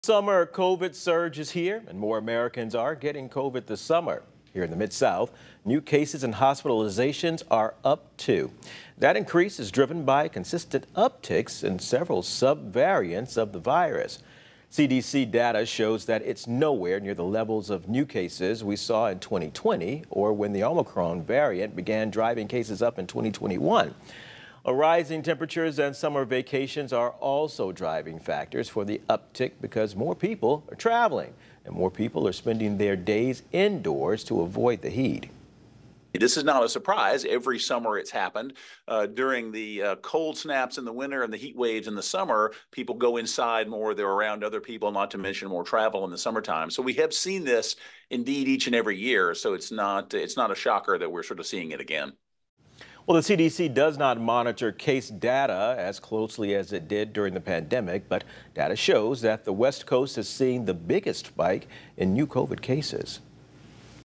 COVID-19 cases and hospitalizations are up this summer in the Mid-South
Rising temperatures and summer vacations are some of the driving factors for the uptick.